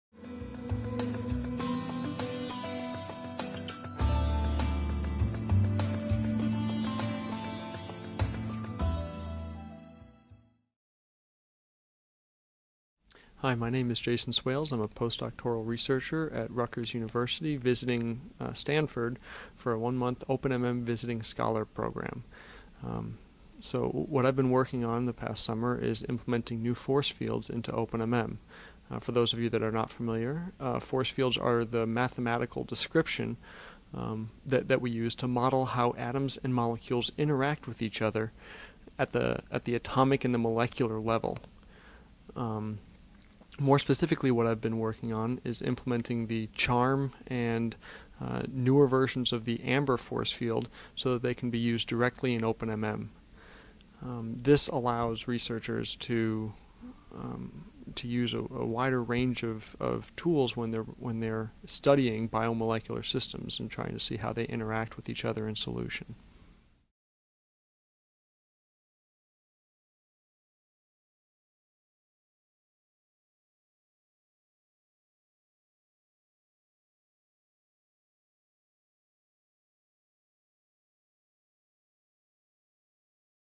Force Fields and Molecular Dynamics
One approach to simulating molecular dynamics is to utilize molecular mechanical force fields as a model for evaluating potential energies and forces on each atom in a molecular system.  In this video, OpenMM Visiting Scholar Jason Swails explains about force fields and his work to expand the support of different force fields within OpenMM.  Read more about biomolecular force fields in Jason's blog posting